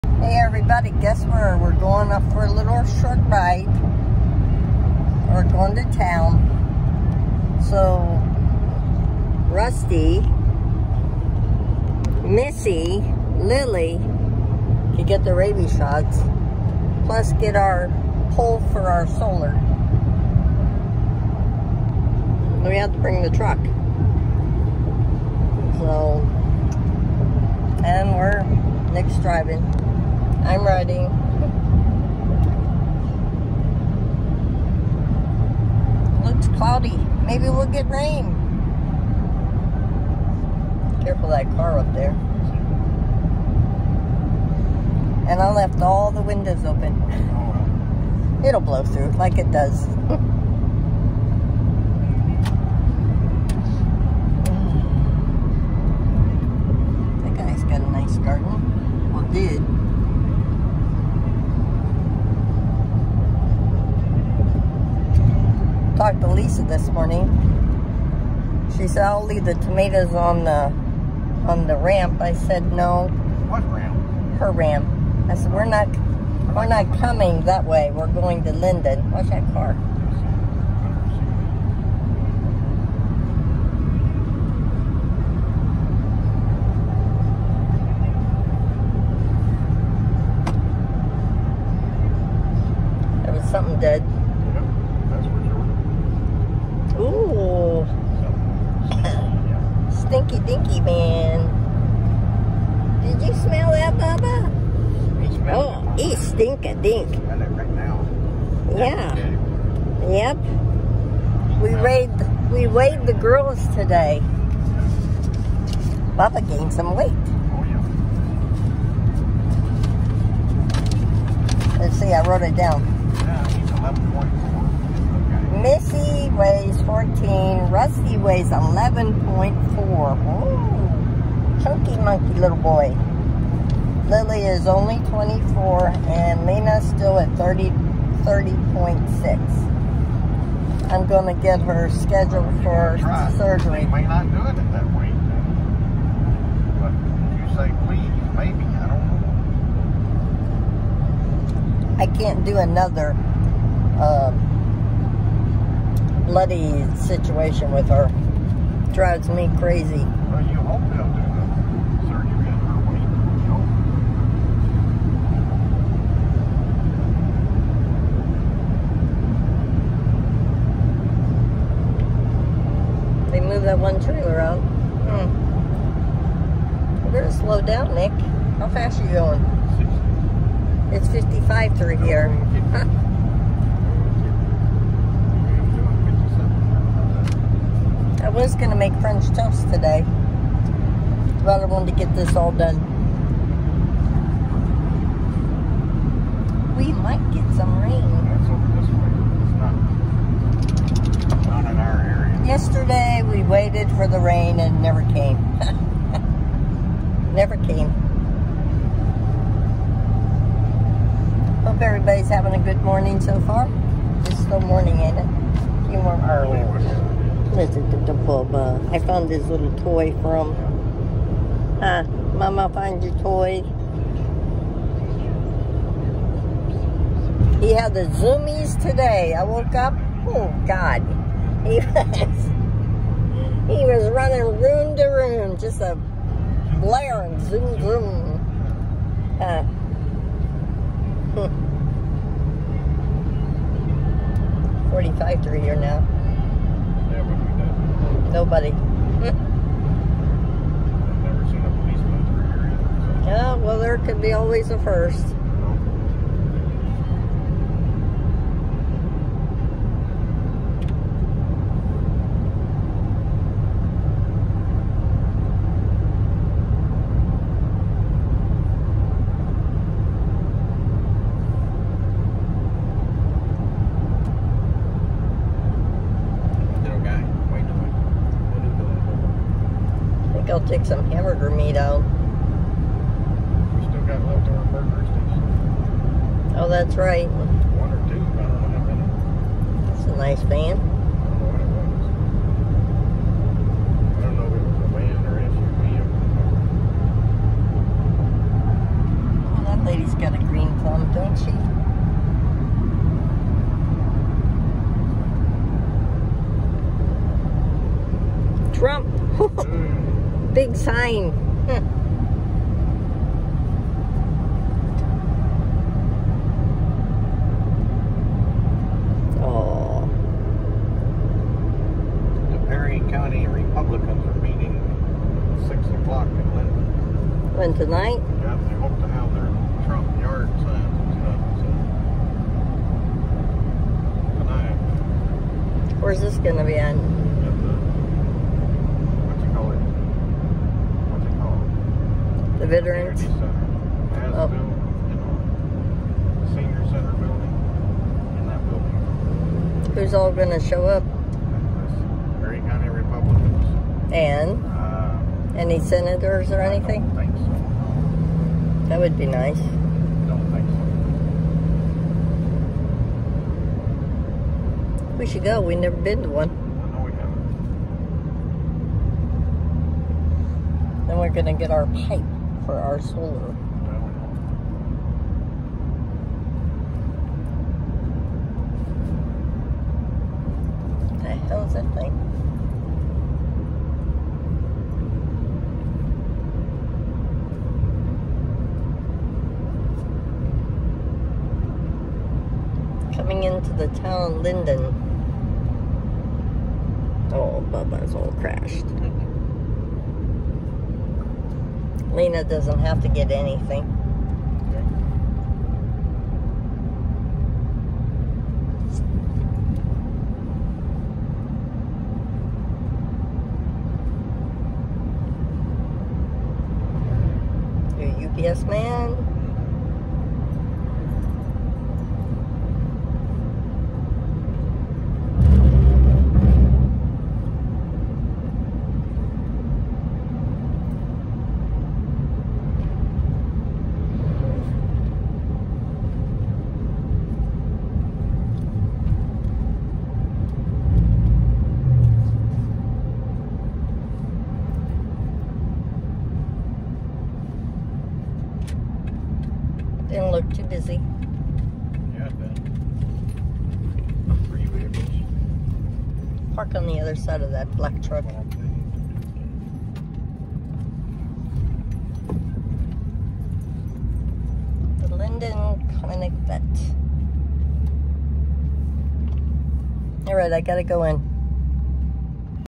July 18, 2024
We taking kids vet & getting pipe the solar now